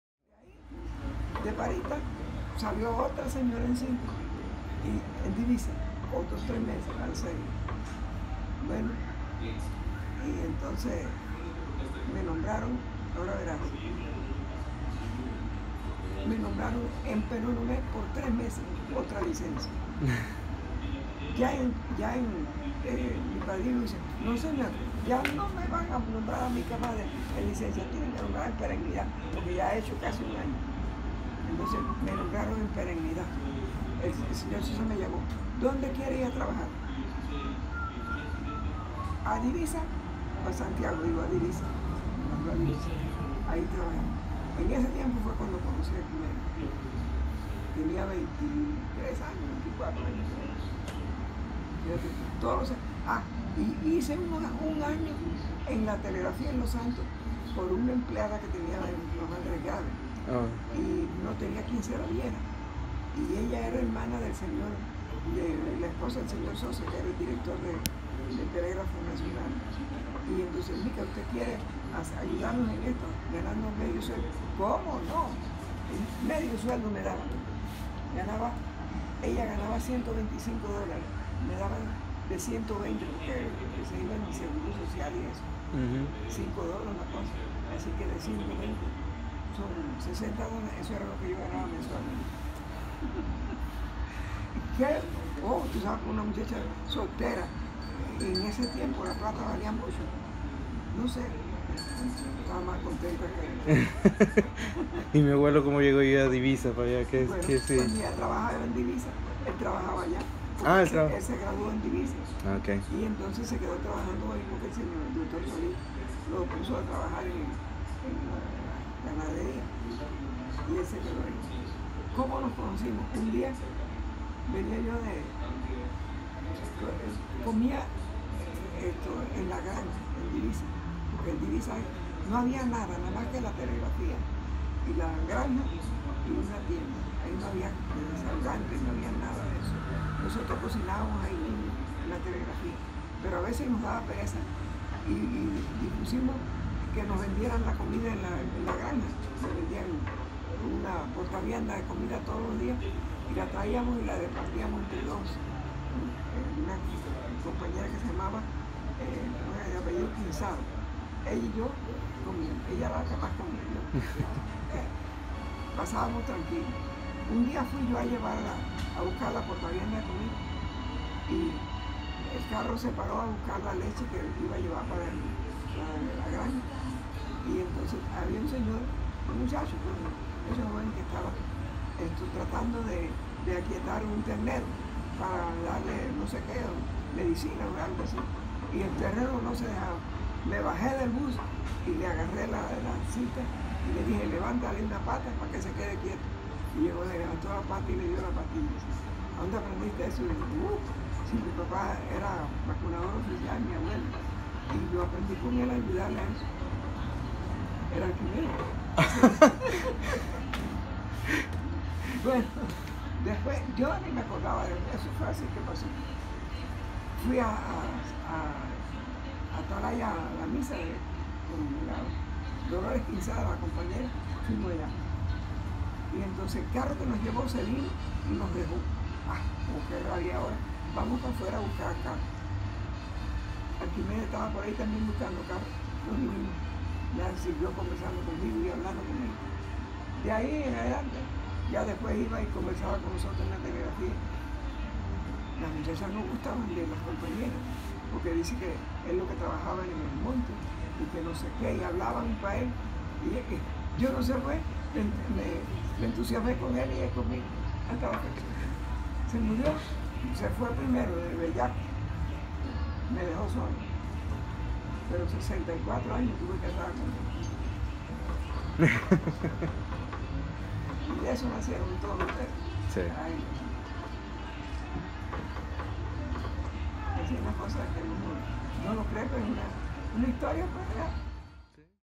Micaela & Arquimedes
Tuve la suerte de guardar la historia de cómo se conocieron mis abuelos...